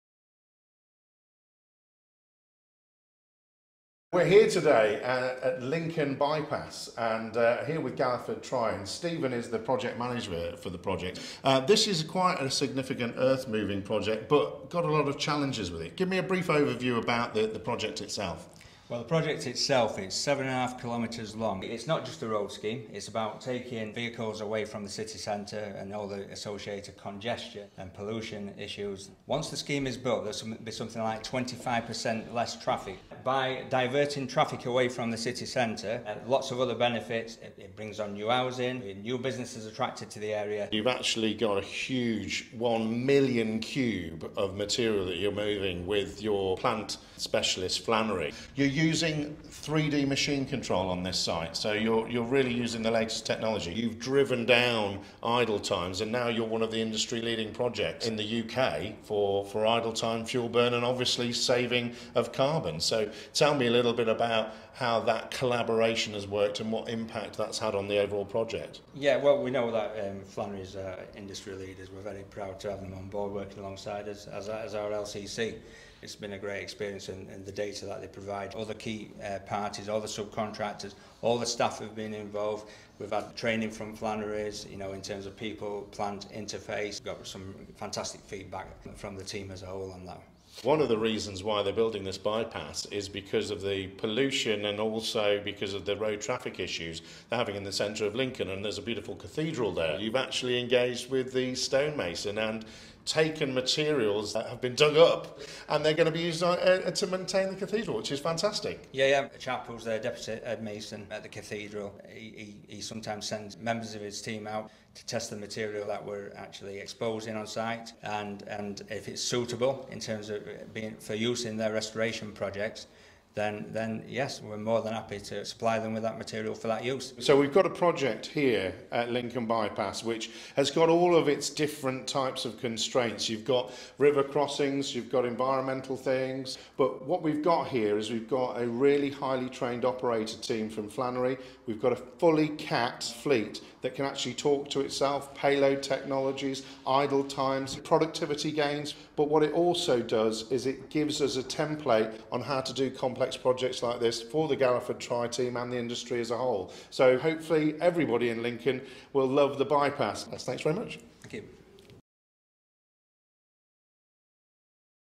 The Lincoln Bypass project with Galiford Try and Flannery Plant Hire
Peter Haddock from Content With Media visited the Lincoln Bypass, an exciting infrastructure project, run by main contractor Galliford Try plc.

They are working with P. Flannery Plant Hire, which is delivering some challenging earthworks for the project, using SITECH UK and Ireland and Trimble Inc. 3D machine control on its Finning supplied Caterpillar fleet.

To find out more about the project, Peter met with the team onsite and recorded this interview with project manager Steven Flanagan.

Peter has also written an article that features in the November 2019 edition of Earthmovers Magazine.